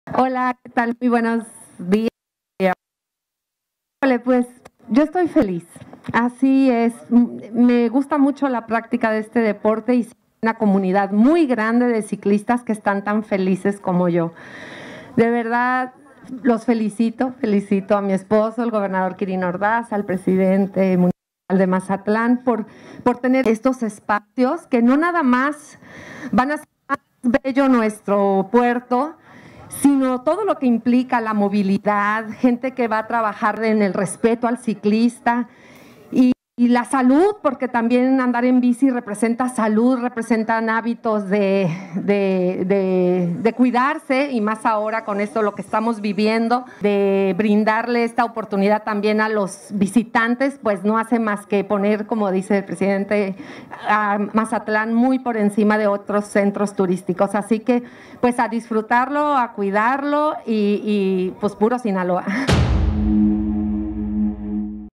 Rosy Fuentes de Ordaz Ciclovía Sábalo Cerritos Mazatlán Zona Trópico 2021
Rosy Fuentes de Ordaz Destaca la Creación de la Ciclovía Sábalo Cerritos en Mazatlán Zona Trópico 2021